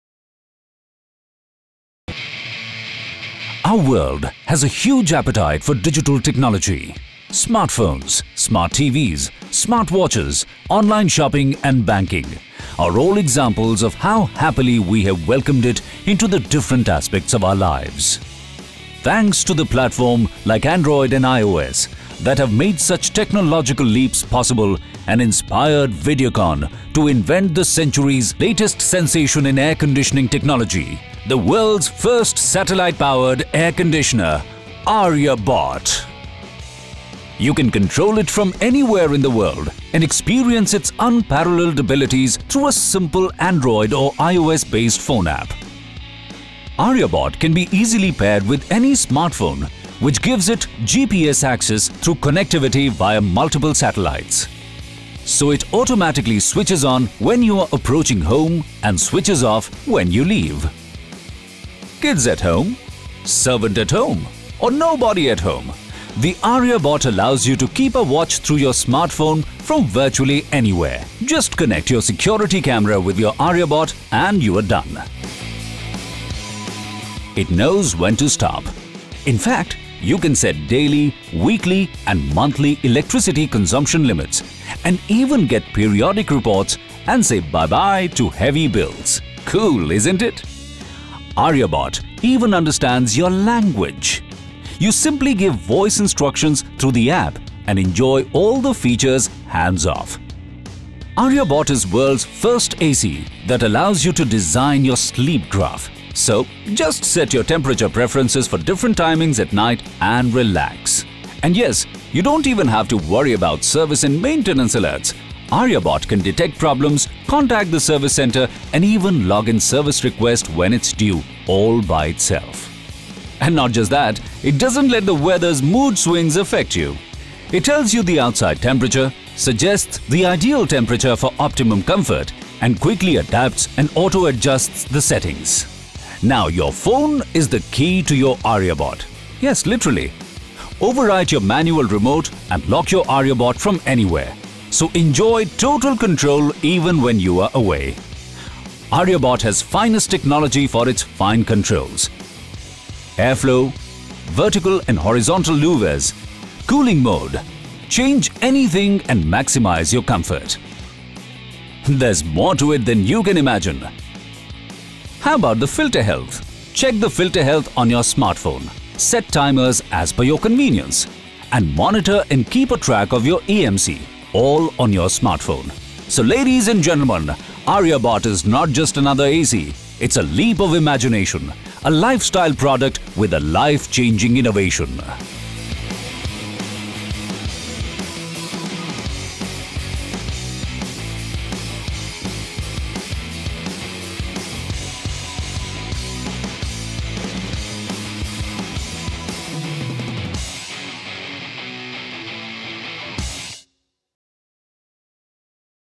Explore Aryabot: India's First Satellite AC
Wifi-enabled and app-based among a plethora of futuristic features, the Aryabot AC will blow you away!